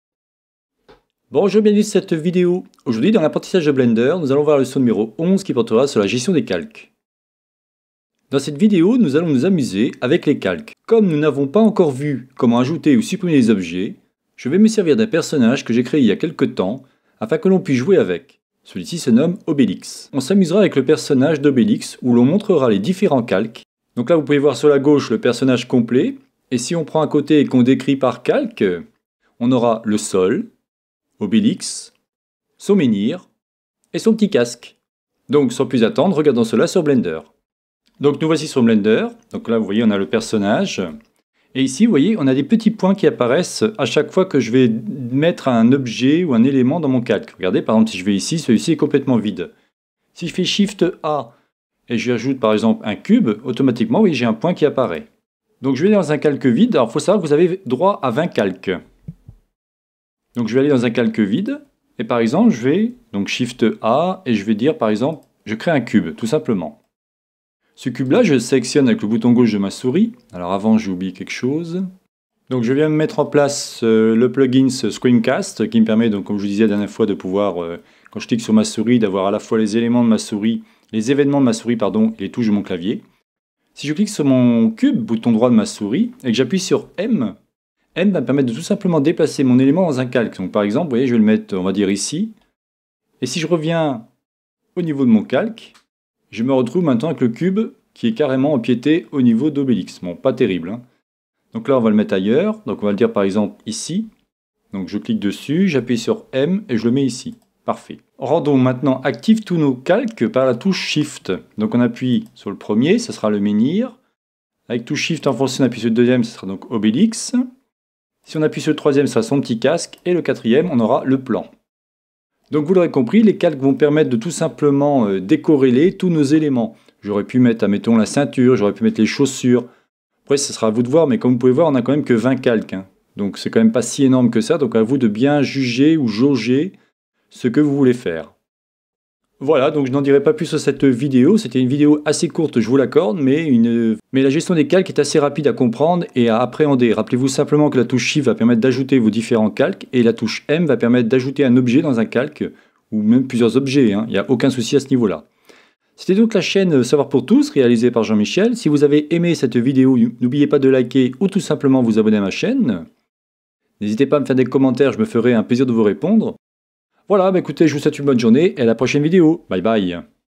Blender - 11 - Calques / Layer
Dans cette vidéo, nous allons apprendre à utiliser les calques sur Blender, en l'occurrence Blender compte 20 calques.
la démonstration se fera avec le personnage Obelix, celui-ci sera composé de 4 calques.